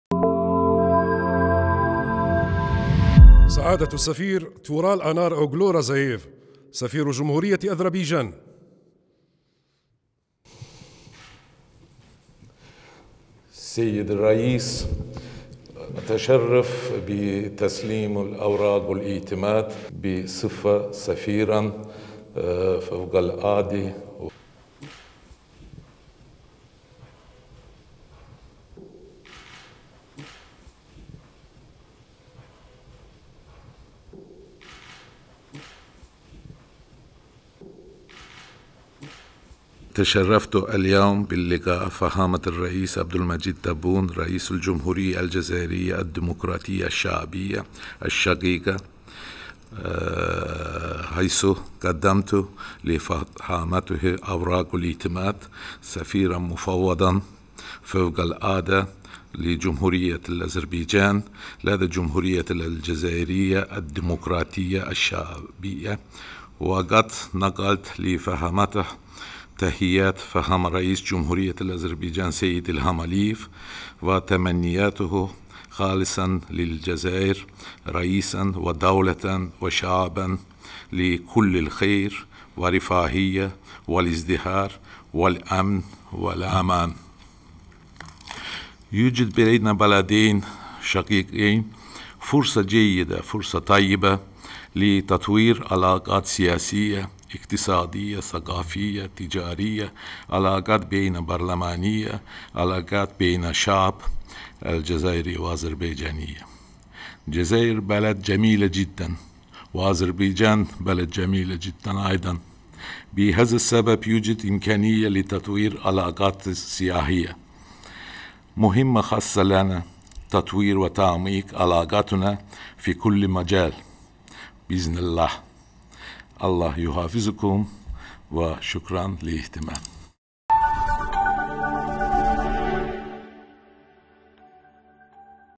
رئيس الجمهورية يستقبل السفير الجديد لجمهورية أذربيجان لدى الجزائر
الجزائر(وأج)- استقبل رئيس الجمهورية، السيد عبد المجيد تبون، السيد تورال أنار أوغلو رزاييف، الذي سلم له أوراق اعتماده بصفته سفيرا جديدا لجمهورية أذربيجان لدى الجزائر، وفي تصريح له عقب  الاستقبال، أكد السفير، أن البلدين يملكان فرصة جيدة لتطوير العلاقات السياسية, الاقتصادية, الثقافية والتجارية وكذا البرلمانية بينهما.